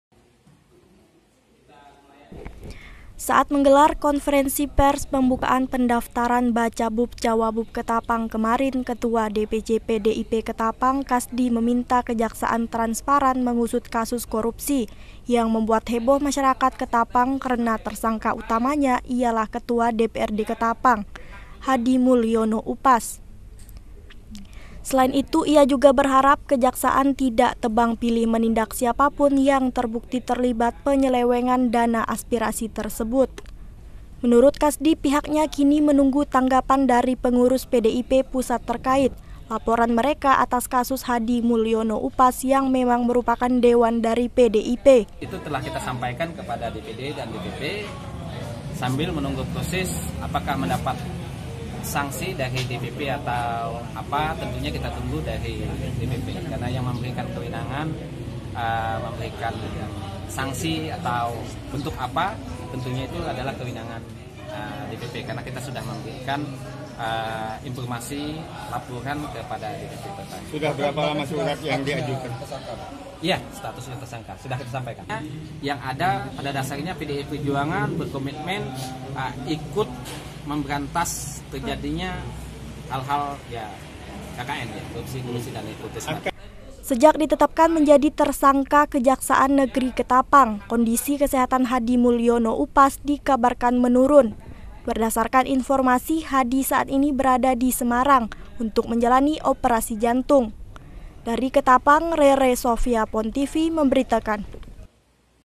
Korupsi Penyelewengan Dana Aspirasi Ketua DPRD Ketapang
Ketua DPC PDIP Ketapang, Kasdi meminta kejaksaan transparan melakukan penyelidikan kasus korupsi Ketua DPRD Ketapang, Hadi Mulyono Upas. Pihaknya juga berharap kejaksaan tidak tebang pilih menindak siapapun yang terbukti terlibat penyelewangan dana aspirasi tersebut